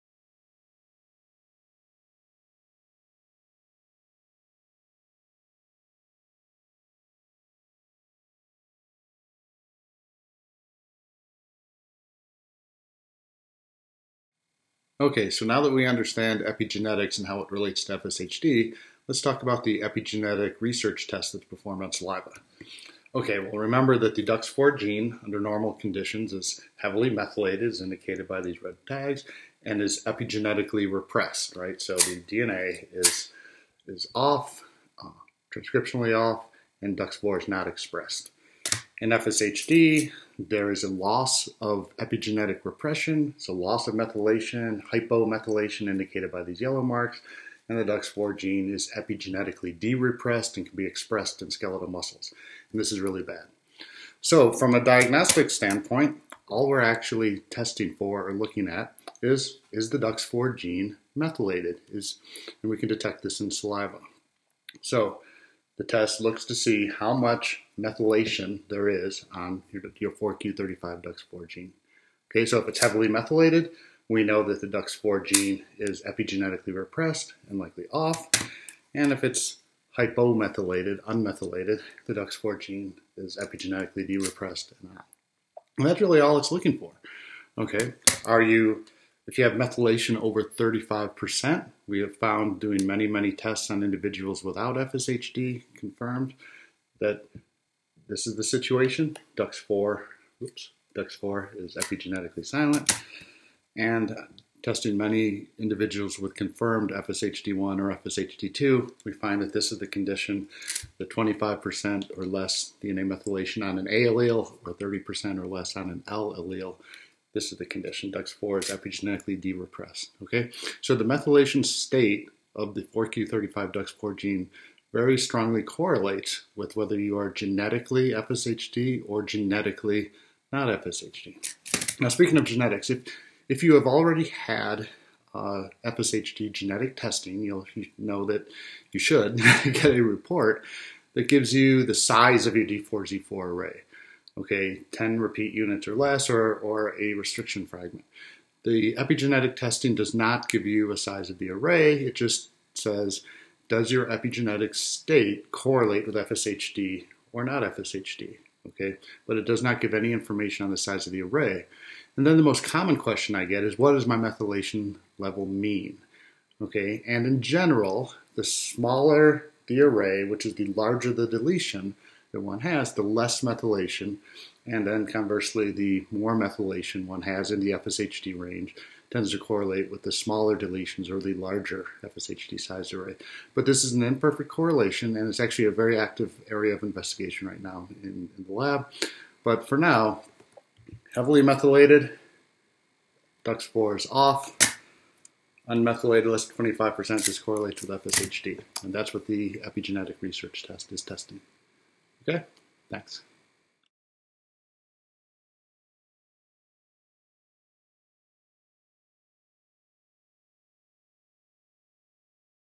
What is FSHD epigenetic research testing?
This saliva-based facioscapulohumeral muscular dystrophy (FSHD) research test identifies the state of your epigenetics to see if it correlates with FSHD or not FSHD. In this video, Dr. Peter L. Jones, PhD, of the Peter and Takako Jones Lab (Reno, Nevada) explains the process.